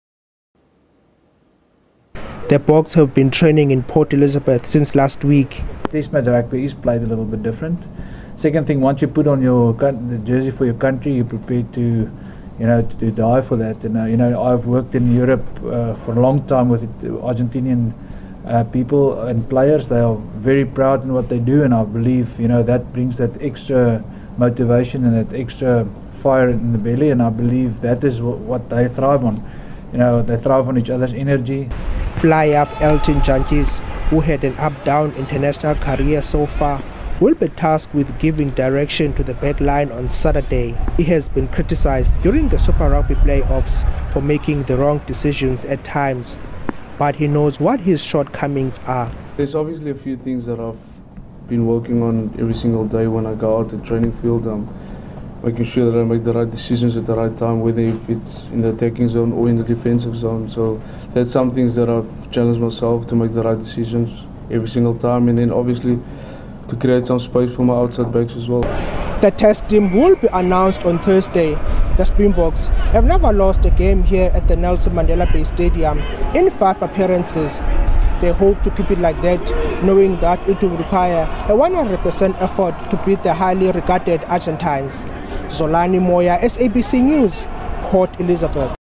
Springboks jumpy ahead of Argentine test
Springbok assistant coach Franco Smith has told his players not to underestimate Argentina in the opening match of the Castle Lager Championships on Saturday at the Nelson Mandela Bay Stadium.
The " Los Pumas' have beaten the Bok's once in three years and they will be aiming to cause an upset. A new game plan is on the cards for the Boks and Smith says players have adapted well. Frans Malherbe has been ruled out of the entire Rugby Championship due to a neck injury but good news is that scrumhalf Francois Hougaard is back in the country and has been training with the Bok's since yesterday.